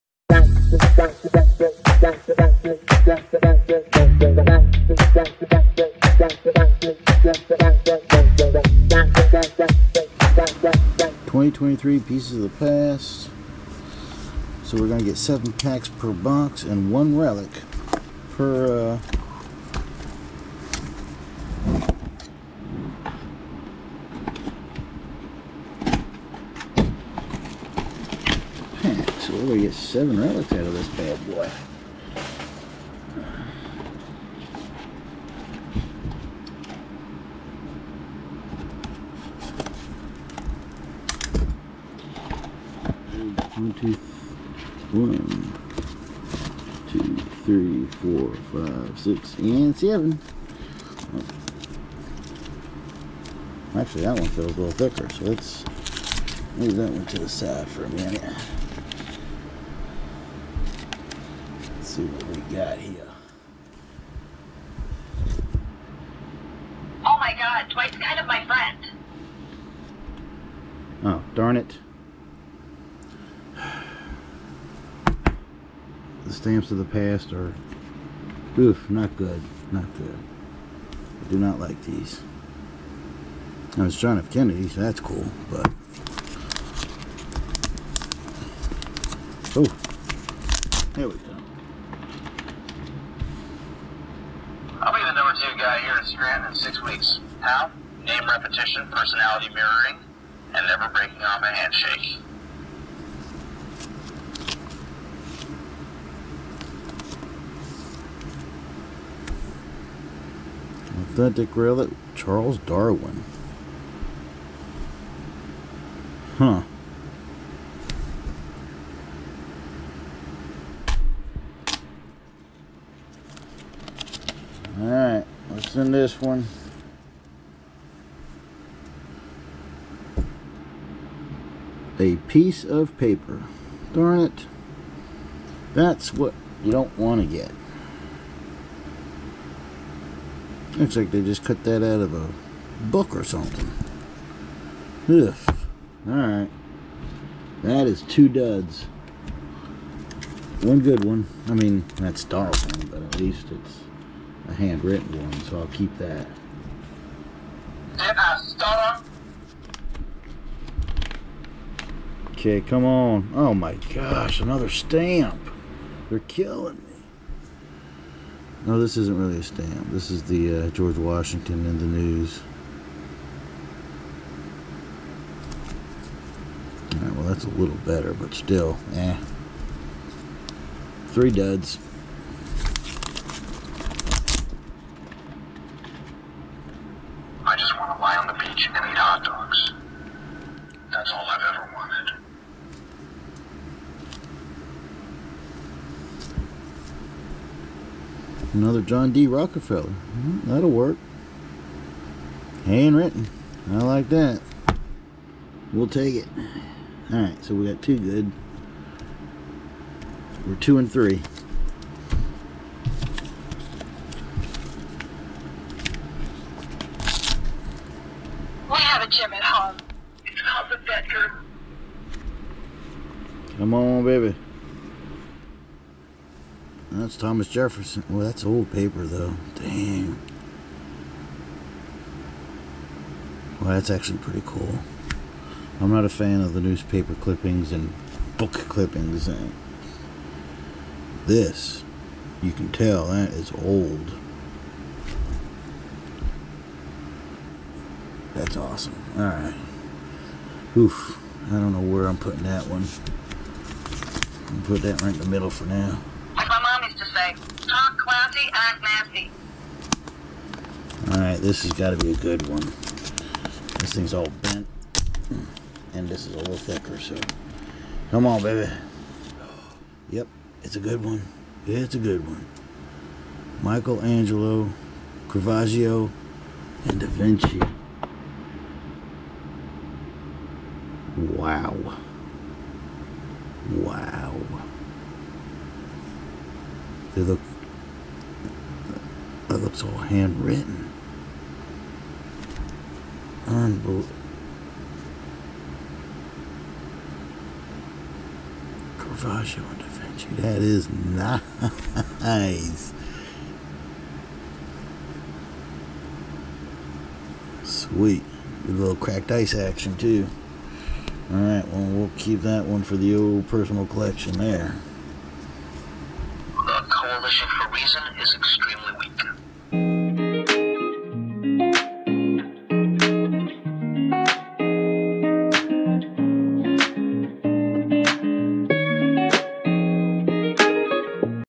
2023 Pieces of the Past 7 Year Collection Box.1 Relic Per Pack 7 Packs/box. Triple Relic SP Pulled.
SUPER PRODUCTS put out a great line of products.
 This is there 7 year collection box. Lets take a look inside.